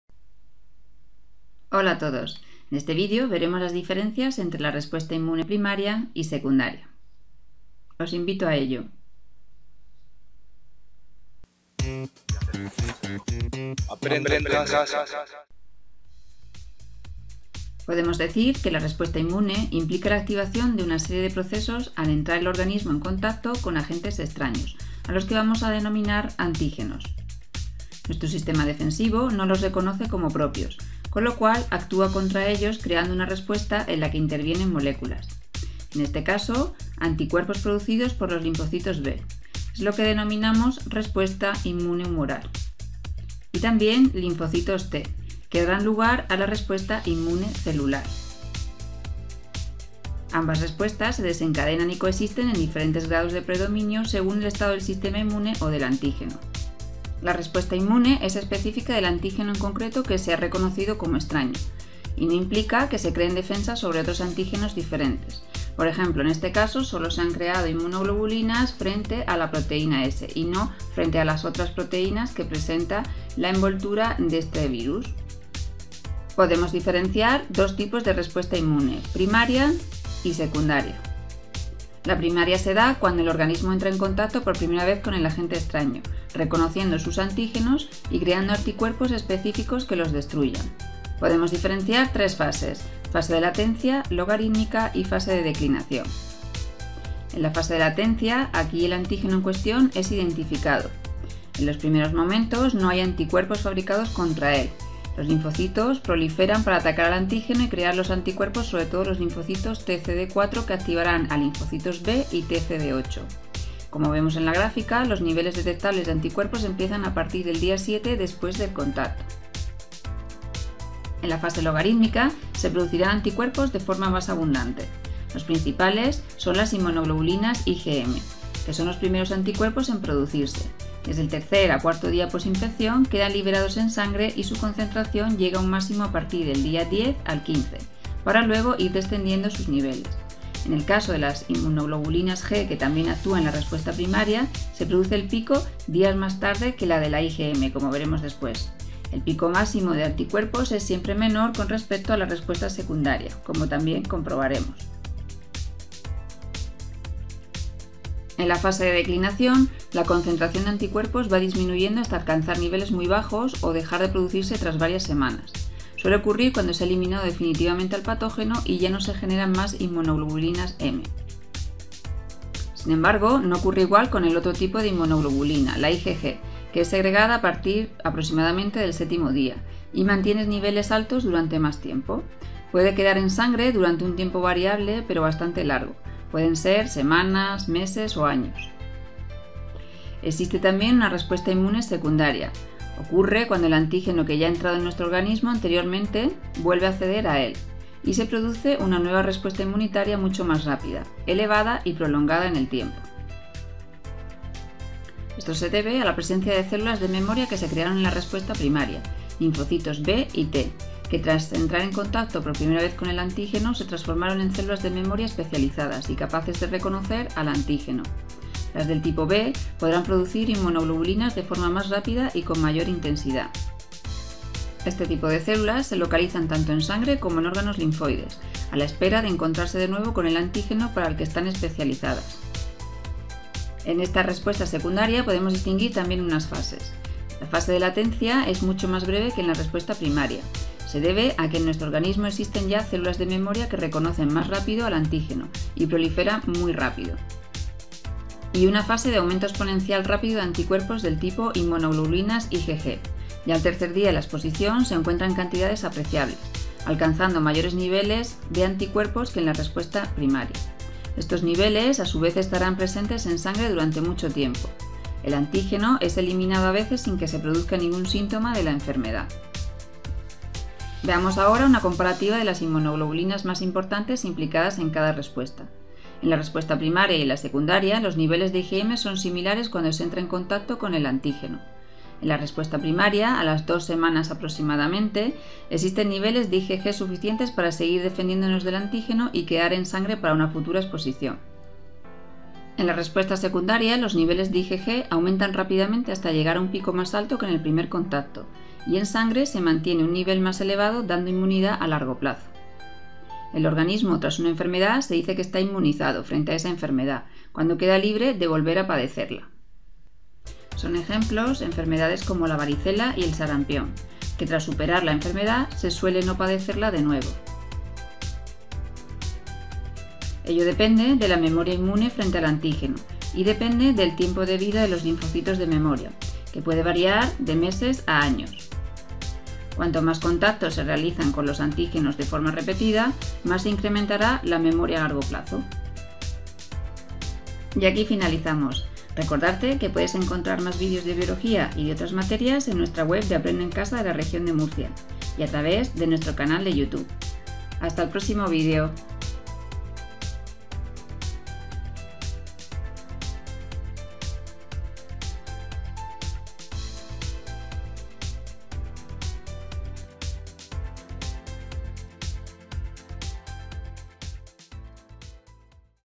RESPUESTA INMUNITARIA PRIMARIA Y SECUNDARIA
El vídeo explica las diferencias entre la  respuesta inmunitaria primaria y  la secundaria.